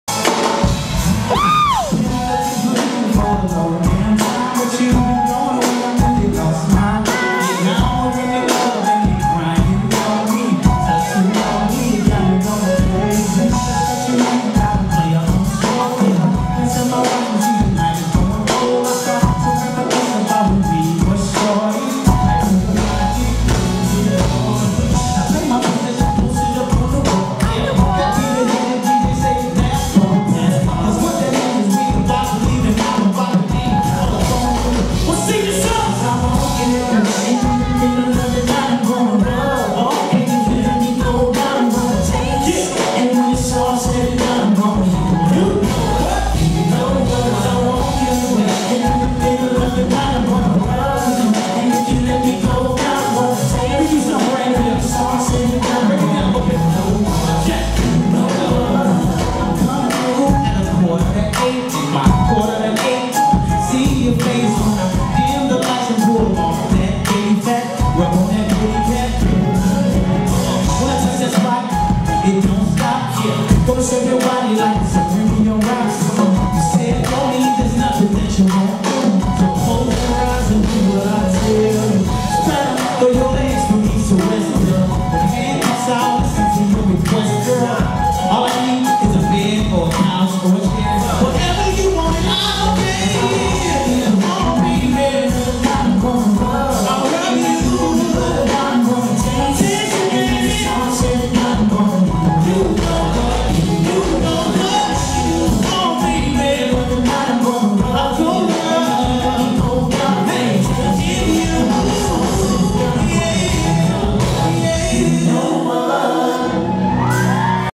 AVANT LIVE
AVANT LIVE... HE IS ALL THAT AND MORE... TRUE R&B..!! (Please excuse my drunk sister's singing hahaaaa)